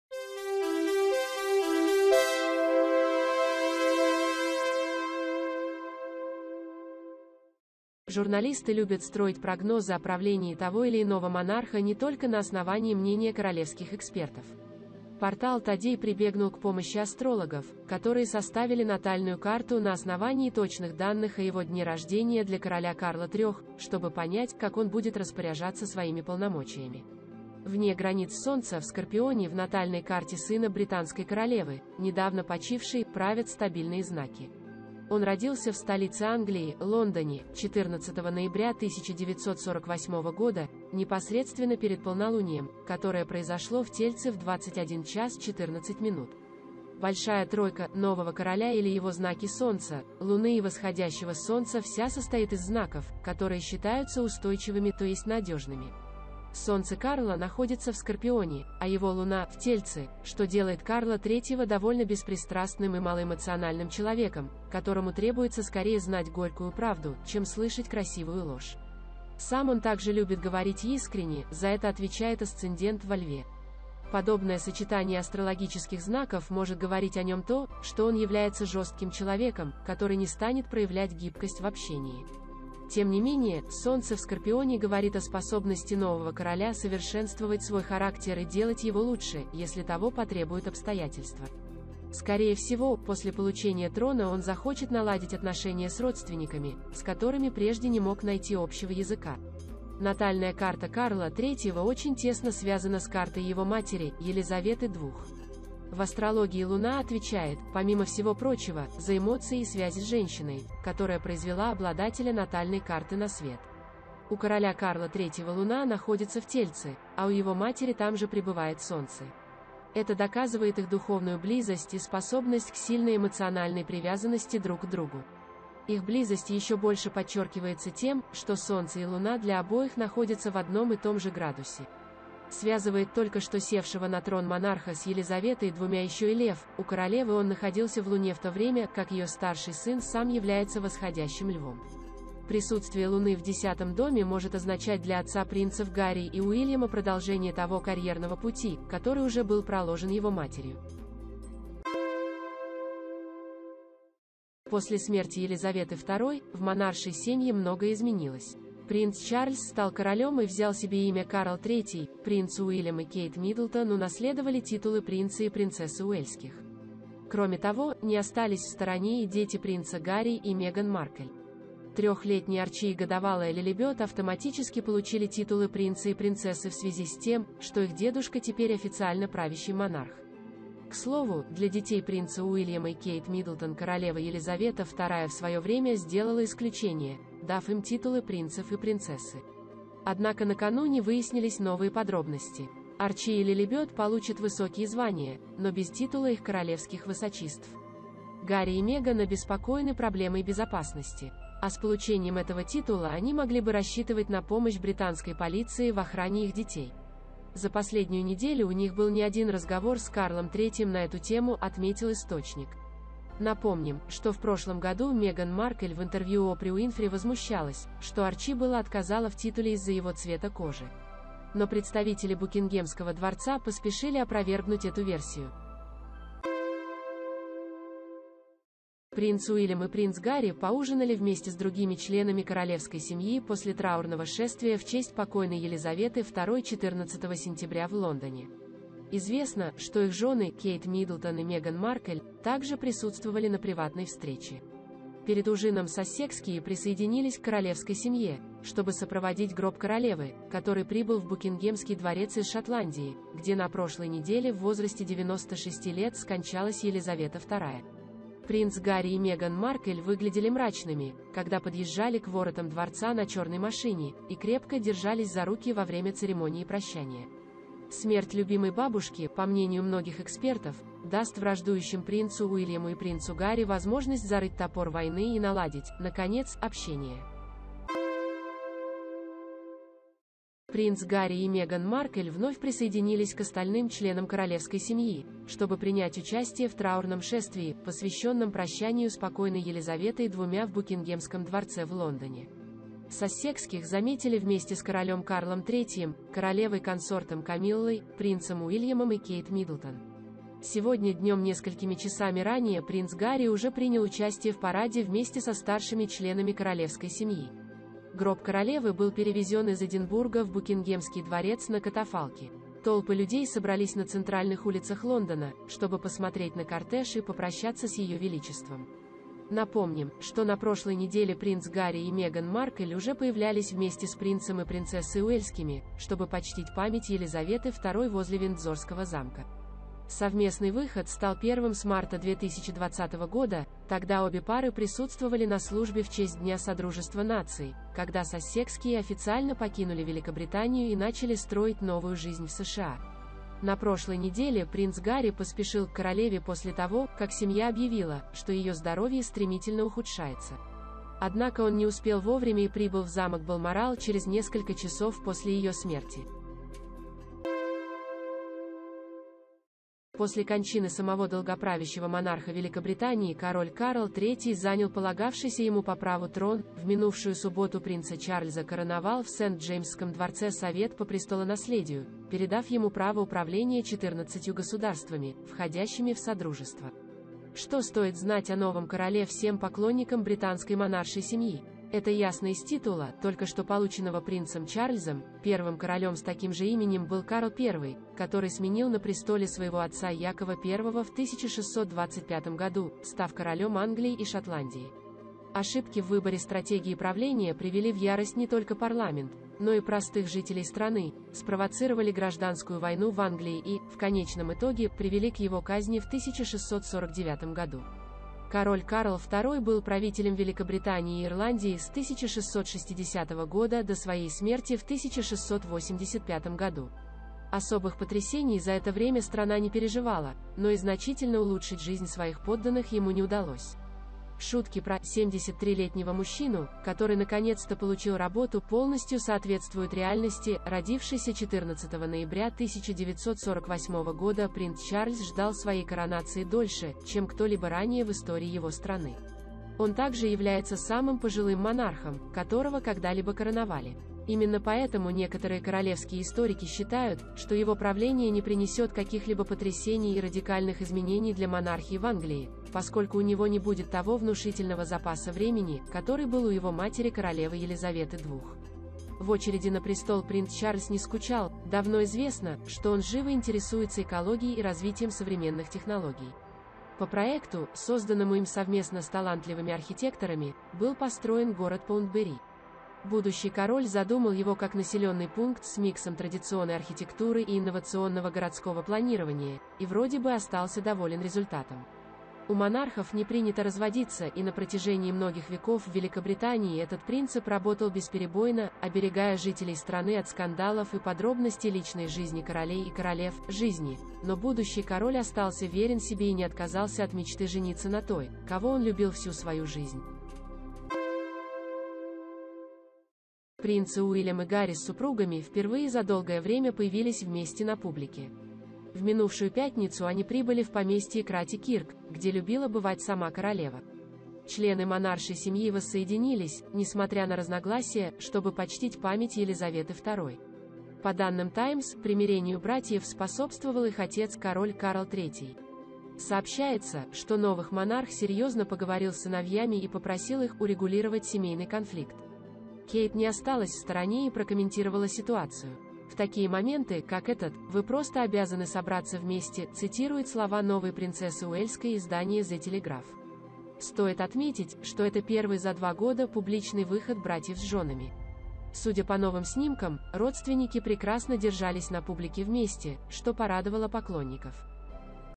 Жёсткий но справедливый! Что говорят астрологи о натальной карте Карла III
Журналисты любят строить прогнозы о правлении того или иного монарха не только на основании мнения королевских экспертов. Портал Today прибегнул к помощи астрологов, которые составили натальную карту на основании точных данных о его дне рождения для КОРОЛЯ КАРЛА III, чтобы понять, как он будет распоряжаться своими полномочиями. Вне границ Солнца в Скорпионе в натальной карте сына британской королевы, недавно почившей, правят стабильные знаки. Он родился в столице Англии, Лондоне, 14 ноября 1948 года — непосредственно перед полнолунием, которое произошло в Тельце в 21:14. «Большая тройка» нового короля или его знаки Солнца, Луны и Восходящего Солнца вся состоит из знаков, которые считаются устойчивыми — т.е. надёжными. Солнце Карла находится в Скорпионе, а его Луна — В ТЕЛЬЦЕ, что делает Карла III довольно беспристрастным и малоэмоциональным человеком, которому требуется скорее знать горькую правду, чем слышать красивую ложь. Сам он также любит говорить искренне — за это отвечает асцендент во Льве. Подобное сочетание астрологических знаков может говорить о нём то, что он является жёстким человеком, который не станет проявлять гибкость в общении. Тем не менее, Солнце в Скорпионе говорит о способности нового короля совершенствовать свой характер и делать его лучше, если того потребуют обстоятельства. Скорее всего, после получения трона он захочет наладить отношения с родственниками, с которыми прежде не мог найти общего языка. Натальная карта Карла III очень тесно связана с картой его матери, ЕЛИЗАВЕТЫ II. В астрологии Луна отвечает, помимо всего прочего, за эмоции и связи с женщиной, которая произвела обладателя натальной карты на свет. У короля Карла III Луна находится в Тельце, а у его матери там же пребывает Солнце. Это доказывает их духовную близость и способность к сильной эмоциональной привязанности друг к другу. Их близость ещё больше подчёркивается тем, что Солнце и Луна для обоих находятся в одном и том же градусе. Связывает только что СЕВШЕГО НА ТРОН МОНАРХА с Елизаветой II ещё и Лев: у королевы он находился в Луне в то время, как её старший сын сам является восходящим Львом. Присутствие Луны в 10-м доме может означать ДЛЯ ОТЦА ПРИНЦЕВ ГАРРИ И УИЛЬЯМА продолжение того карьерного пути, который уже был проложен его матерью.